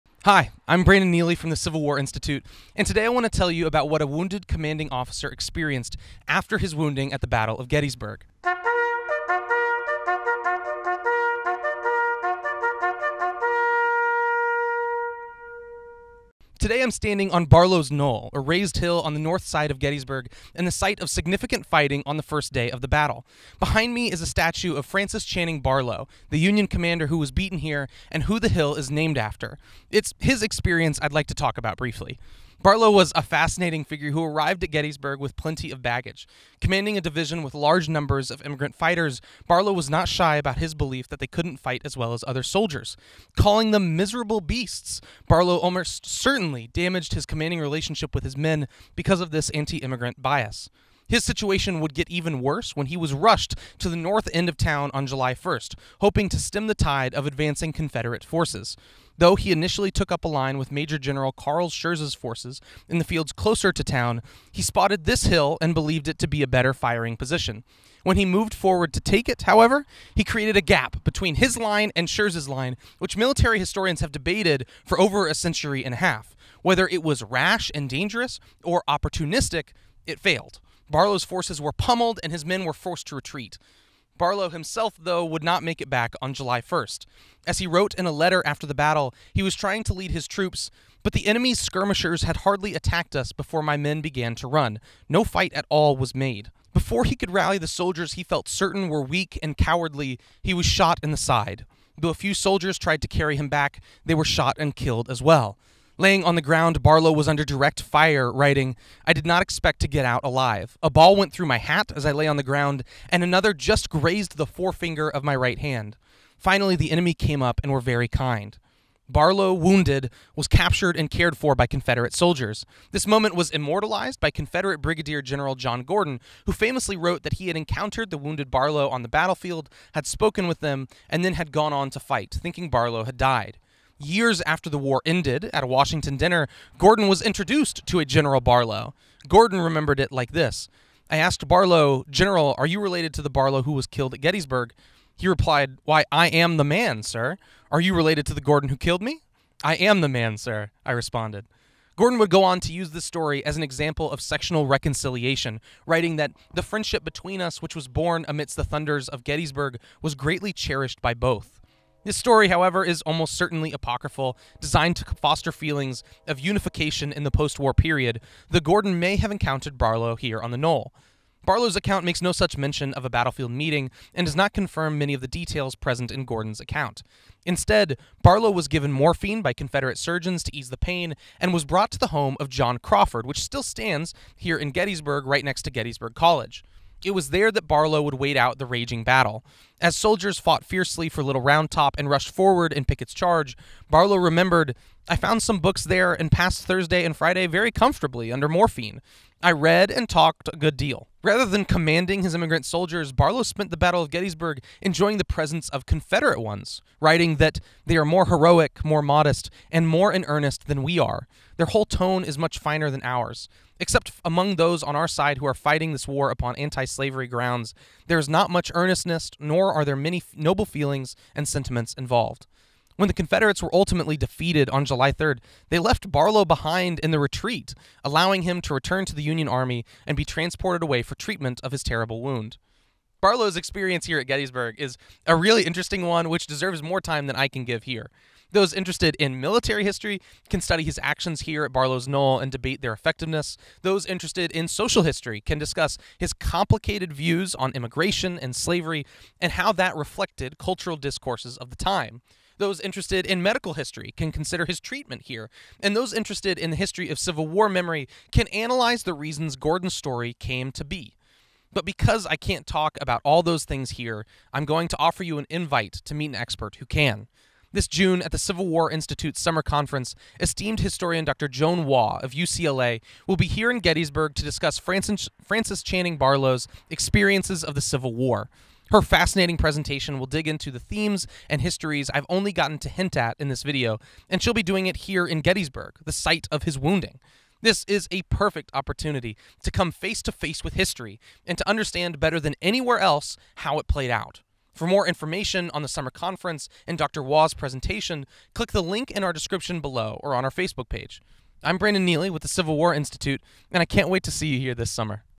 After the Wound: Francis Barlow at Gettysburg
On July 1st, 1863, Francis Channing Barlow was shot on the Gettysburg battlefield. In the days to come, he would rest in the company of Confederate soldiers before being abandoned to his own lines. In this video, CWI Fellow Brandon Neely '23 gives an overview of Barlow's Gettysburg experience and highlights some key events of interest.

The story of Francis Barlow will be elaborated on in more detail by Dr. Joan Waugh at our CWI Summer Conference.